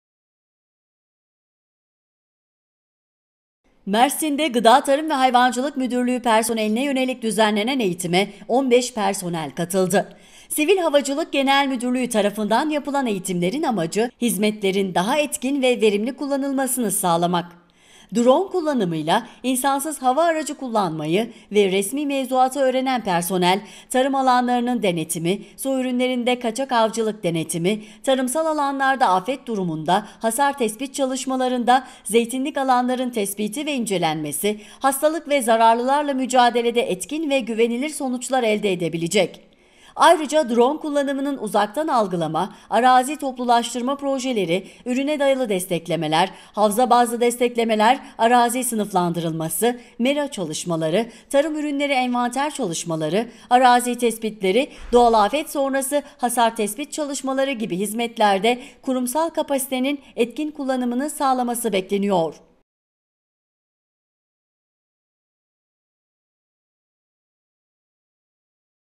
Tarımla teknolojinin buluşması
Mersin İl Gıda Tarım ve Hayvancılık Müdürlüğü personeline drone kullanma eğitimi verildi.
Tarımla teknolojinin buluşmasıHaber: Kahramanmaraş İl Enformasyon – Erdem BAĞCI
Kamera: Mehmet GÜNDÜZ
Mersin’de Gıda Tarım ve Hayvancılık Müdürlüğü personeline yönelik düzenlenen  eğitime 15 personel katıldı.
Sivil Havacılık Genel Müdürlüğü tarafından yapılan eğitimlerin amacı hizmetlerin daha etkin ve verimli kullanılmasını sağlamak.
Drone kullanımı ile insansız hava aracı kullanmayı ve resmi mevzuatı öğrenen personel, tarım alanlarının denetimi, su ürünlerinde kaçak avcılık denetimi, tarımsal alanlarda, afet durumunda, hasar tespit çalışmalarında, zeytinlik alanların tespiti ve incelenmesi, hastalık ve zararlılarla mücadelede etkin ve güvenilir sonuçlar elde edebilecek.
Ayrıca drone kullanımının uzaktan algılama, arazi toplulaştırma projeleri, ürüne dayalı desteklemeler, havza bazlı desteklemeler, arazi sınıflandırılması, mera çalışmaları, tarım ürünleri envanter çalışmaları, arazi tespitleri, doğal afet sonrası hasar tespit çalışmaları gibi hizmetlerde kurumsal kapasitenin etkin kullanımını sağlaması bekleniyor.